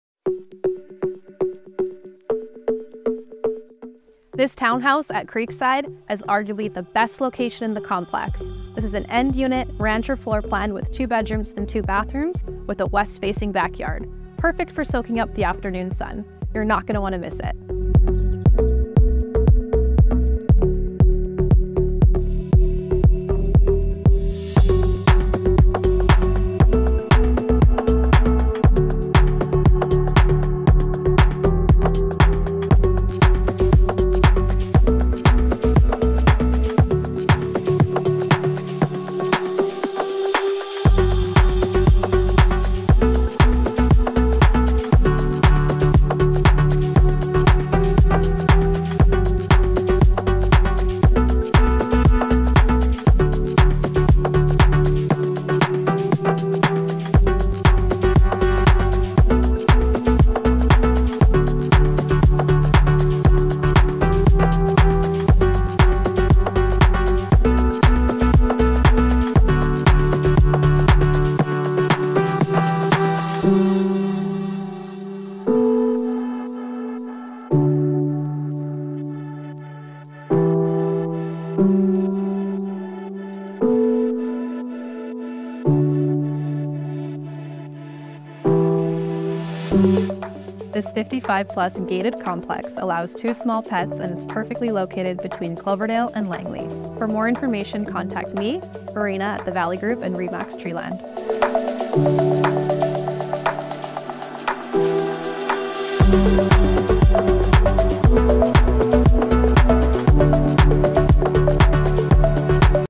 You HAVE to see this backyard! 55+ Rancher at CREEKSIDE | Property Tour: #16 6885 184 St, Cloverdale
In this video Marina with the Valley Group showcases our latest listing in Cloverdale. A wonderful END UNIT rancher at Creekside at Clayton Hills.

MLS# R2880678

One of Cloverdale's coveted gated communities. #16 has the best location in the complex-private END UNIT Rancher offering 2 bedrooms & 2 full bathrooms across 1,400 SQFT. Relax in your home that feels like new w/updated laminate floors, shaker cabinets & 2 living areas w/cozy gas fireplace. The primary ensuite inclds tub, separate shower, his/hers sinks plus walk-in closet. Bask in the afternoon sun in the west-facing backyard with new vinyl fencing, gas BBQ outlet & patio. Other features: new furnace (Jan 2023), roof (2020), roadways (2023), double garage & driveway w/visitor out front. Located minutes from Hillcrest Village Shopping Center & perfectly situated between Langley & Cloverdale. One owner must be 55+, 2 Pets max 16".

The Valley Group
RE/MAX Treeland Realty